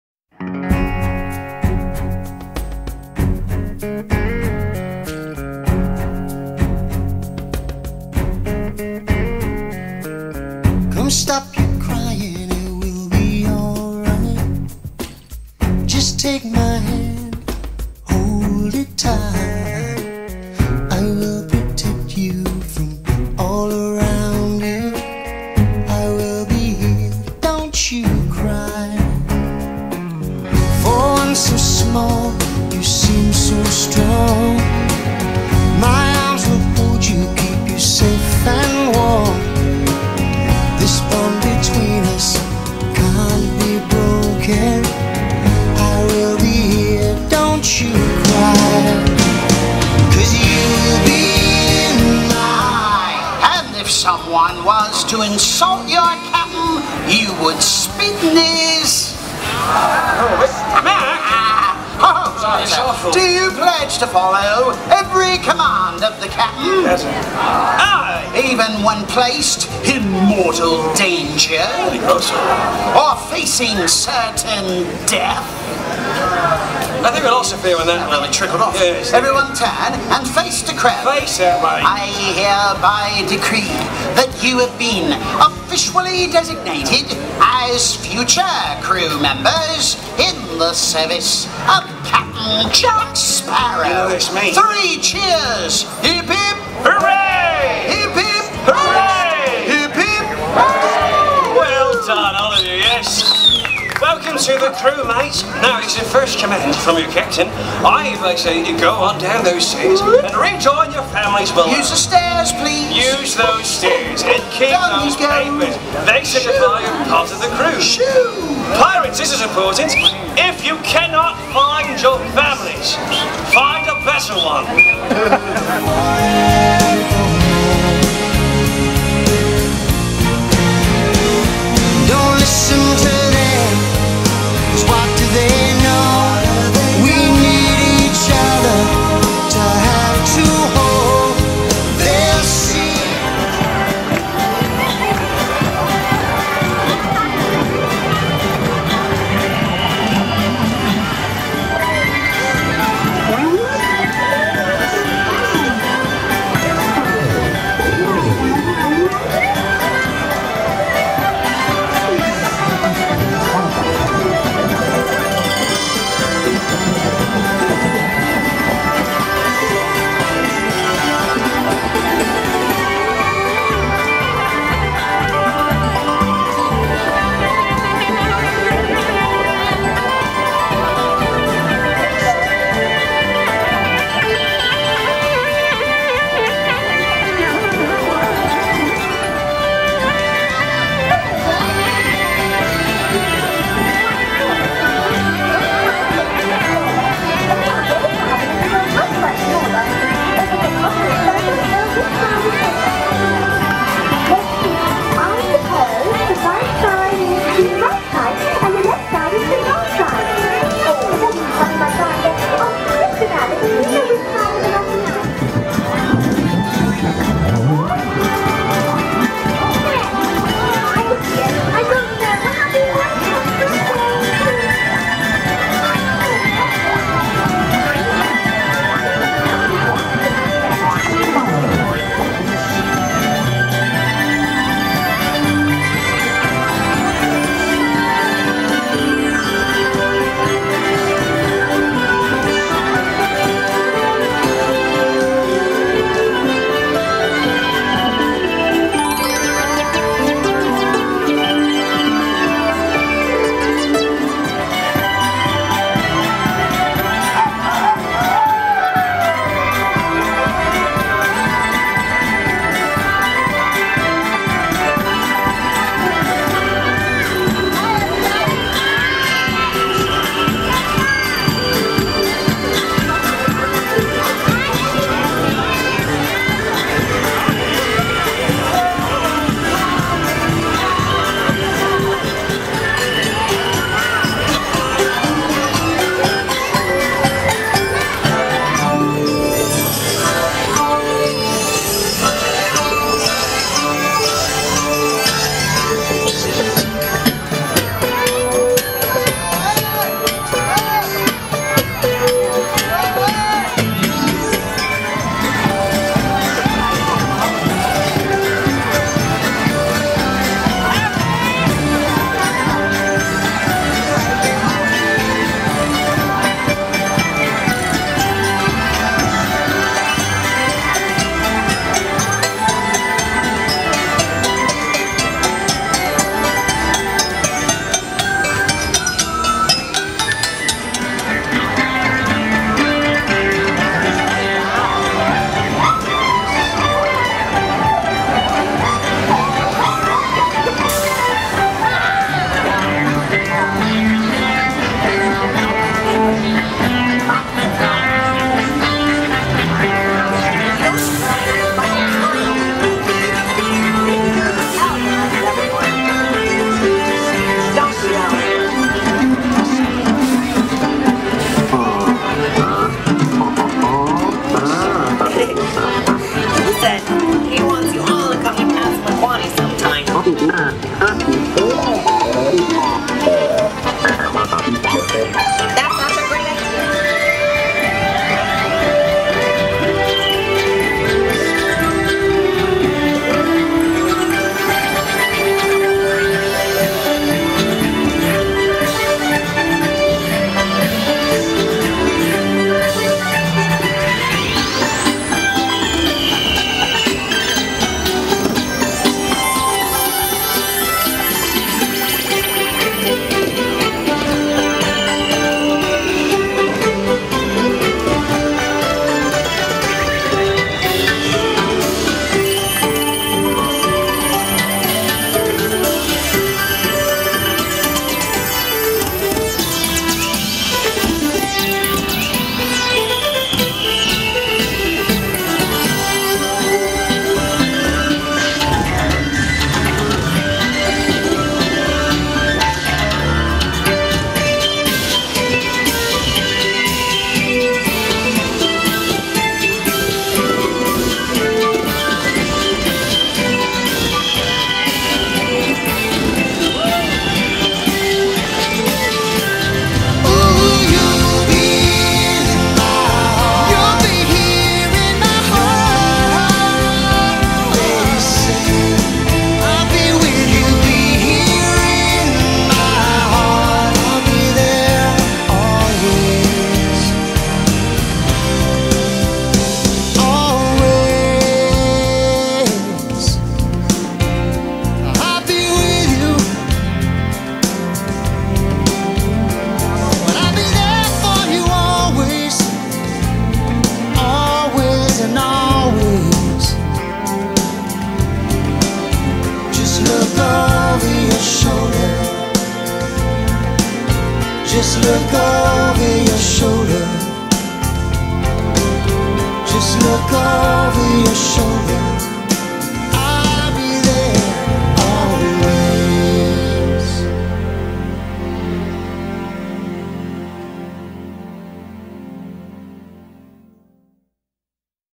Day Three Mariam at Disney
A snapshot of a day of exploring the park.